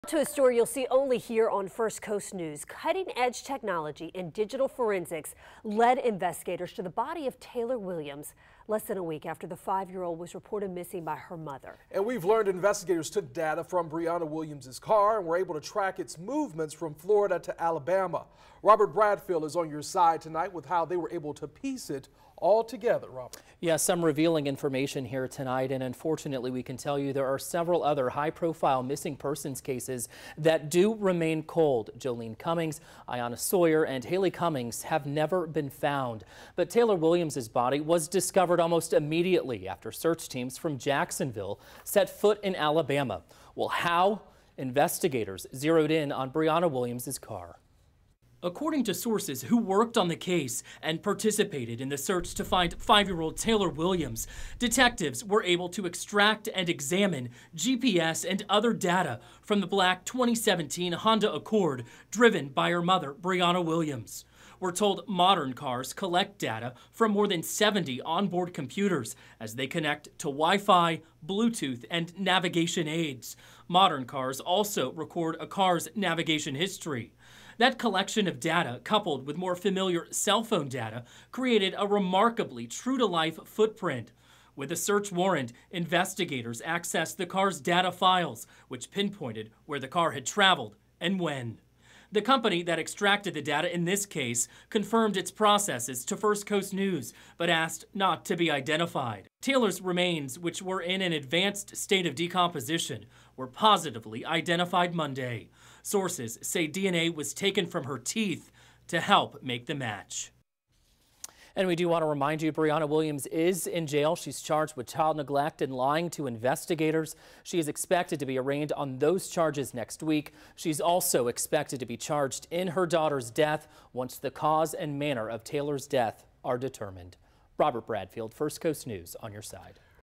Cutting-edge technology led investigators to body of Taylor Williams in Alabama
Cutting-edge digital forensics allowed investigators to glean historical data from Brianna Williams’ car to learn where she’d been.